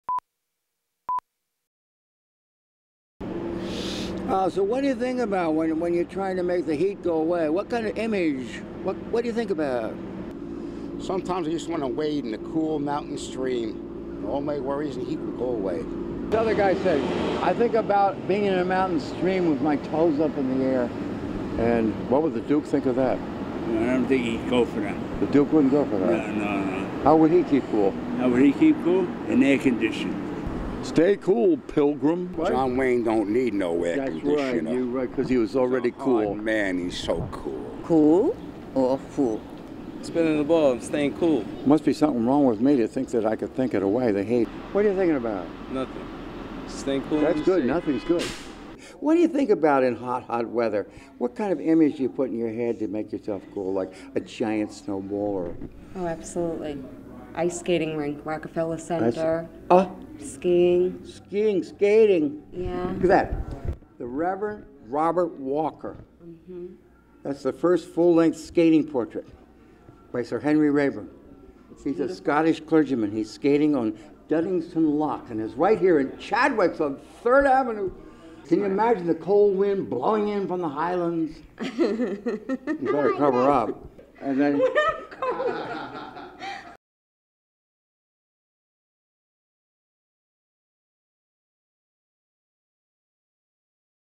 Chauncey Howell thinks Heat is in The Head
So, think cool thoughts about cool things, especially Raeburn's famous picture of  The Skating Minister (1791) cooling the patrons at Chadwick's, delightful watering hole in BayRidge, Brooklyn